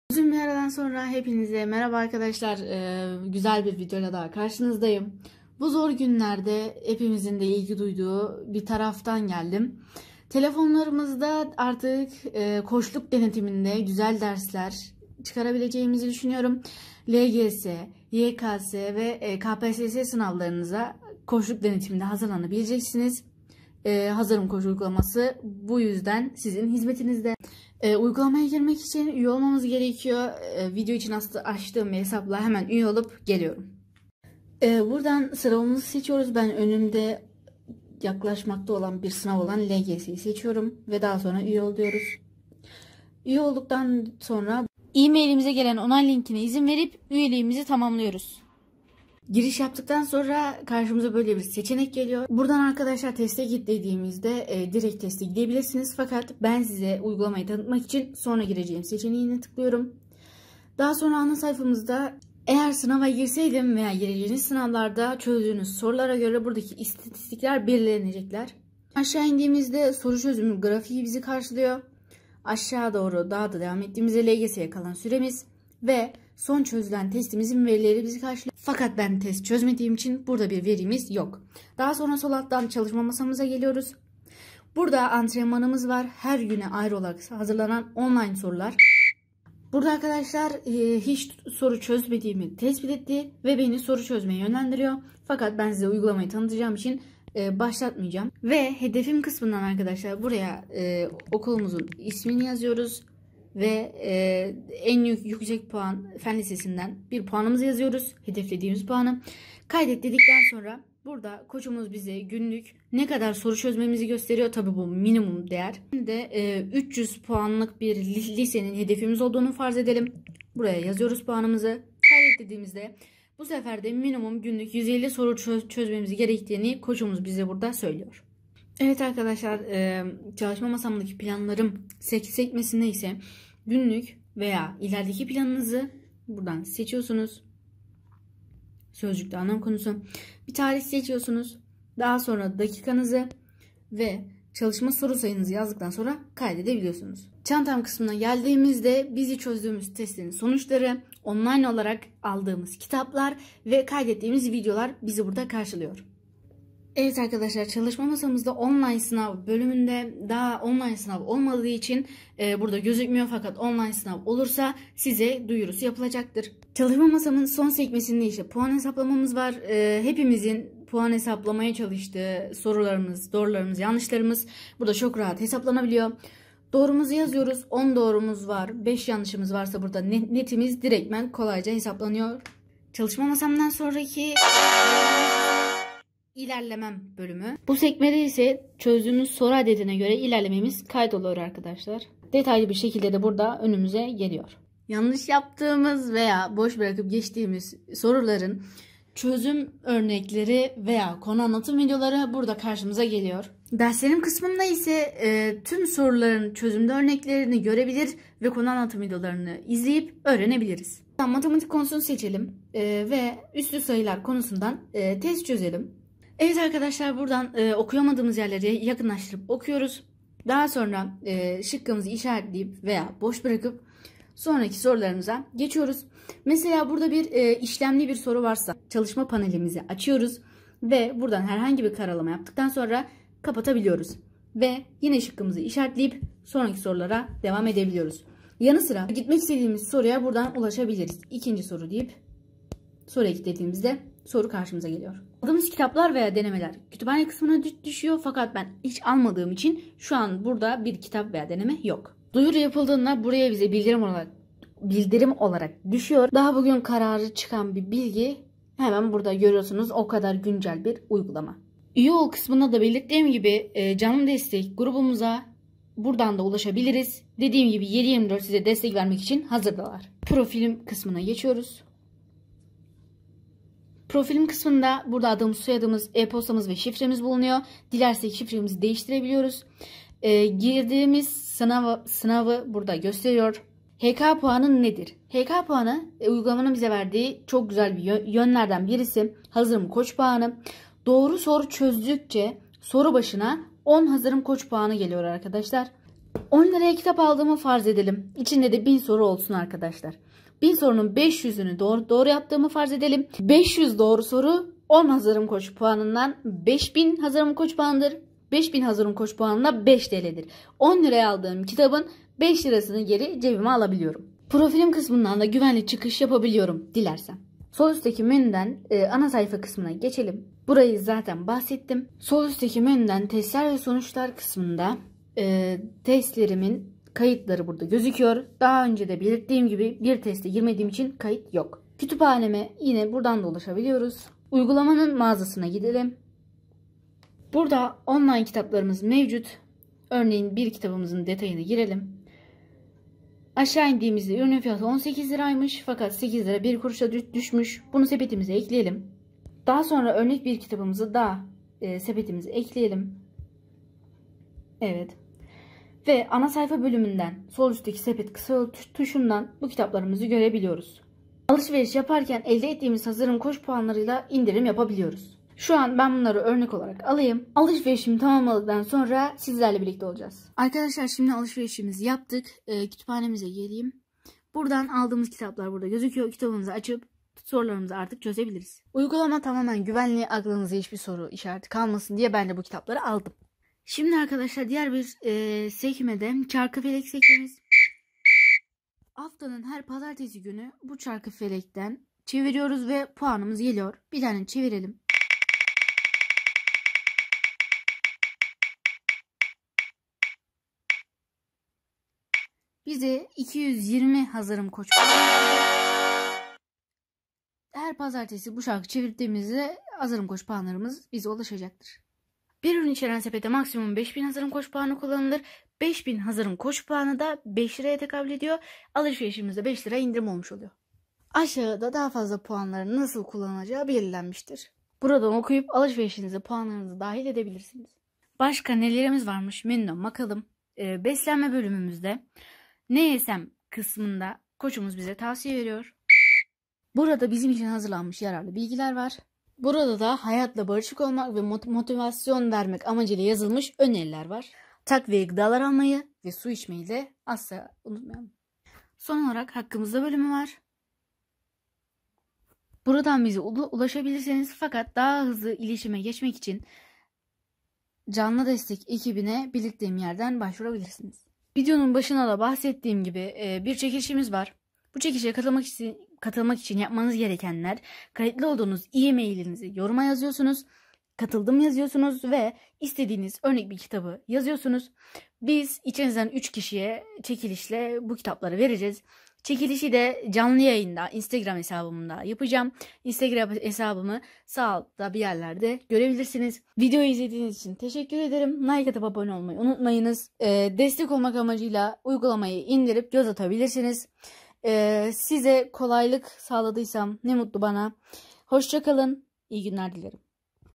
HAZIRIM KOÇ LGS - YKS - KPSS UYGULAMASI DETAYLI TANITIM - ÇEKİLİŞ VAR!!!
Merhaba ben Talha KARAGÖZ. Bugün sizlere Hazırım Koç Uygulaması nın tanıtımını yaptım. Çekilişe katılanların arasından 3 kişiye ücretsiz kitap hediye edilecektir. Bu fırsatı kaçırmayın. Umarım bu zor günlerde size yardımcı olabilmişizdir.
Şuanlık AppStore' da yokuz. IOS kullanıcıları web sitemizden ulaşabilirler.